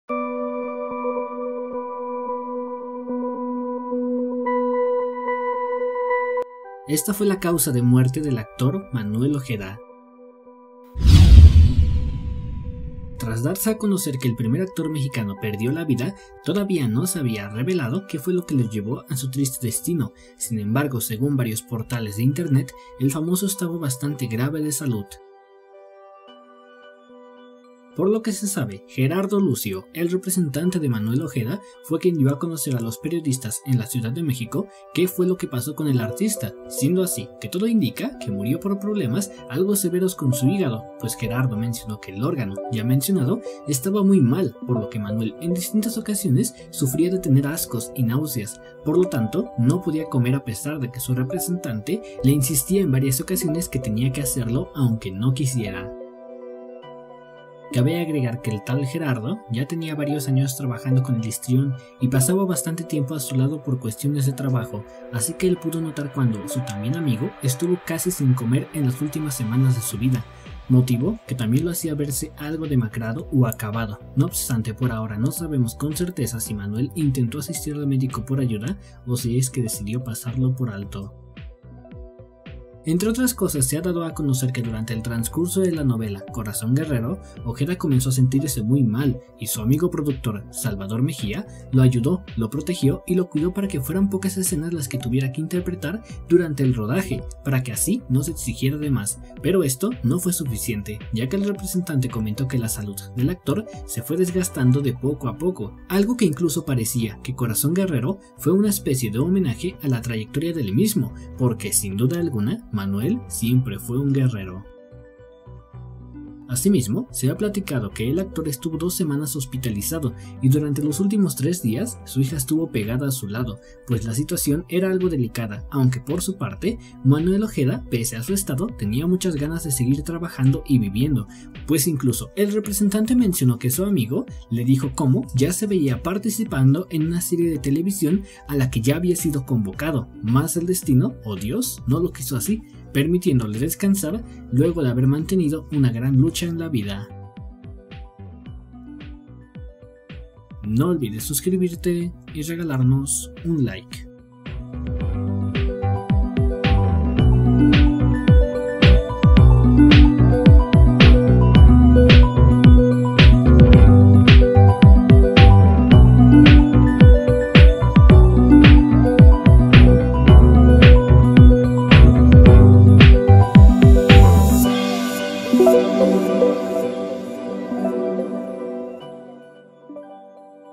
“ Ya estaba muy mal” SE REVELAN LAS CAUSAS DE MUERTE de Manuel Ojeda.
Esta fue la causa de muerte del actor Manuel Ojeda